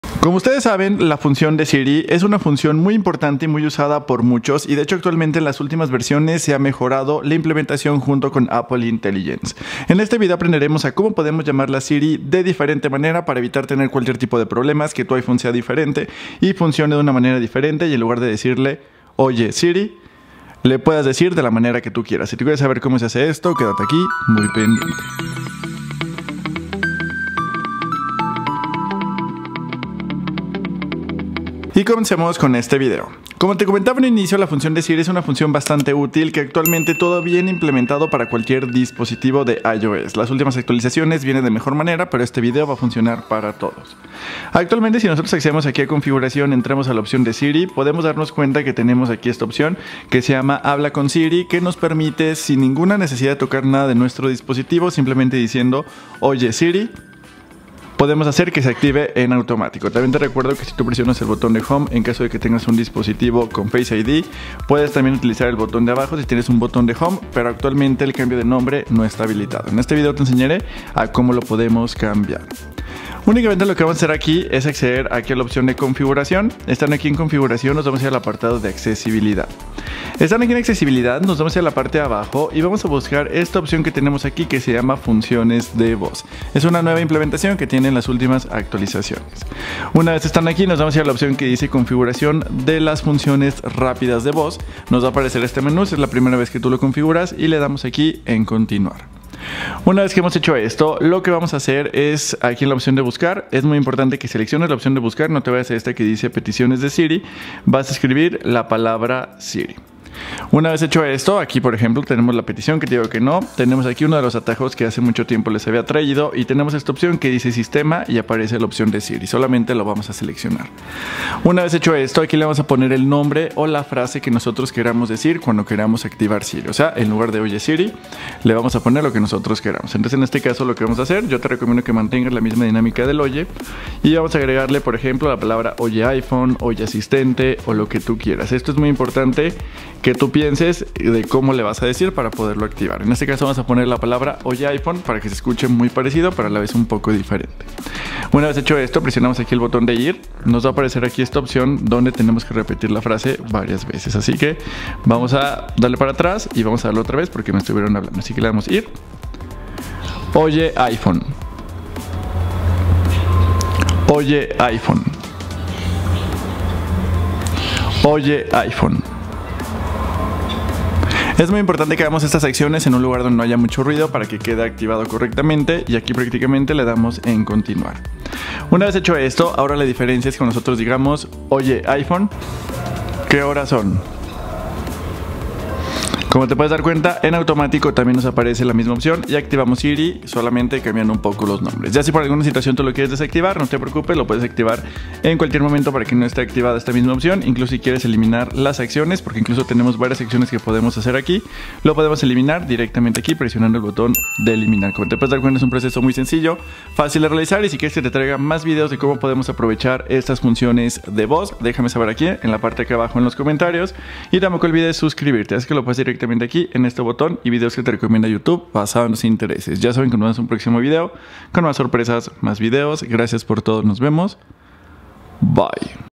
TRUCO | HABLALE a SIRI con OTRO NOMBRE
En este video te traemos un truco de como podemos cambiar el nombre a Siri para poderlo llamada de manera diferente como nosotros queramos.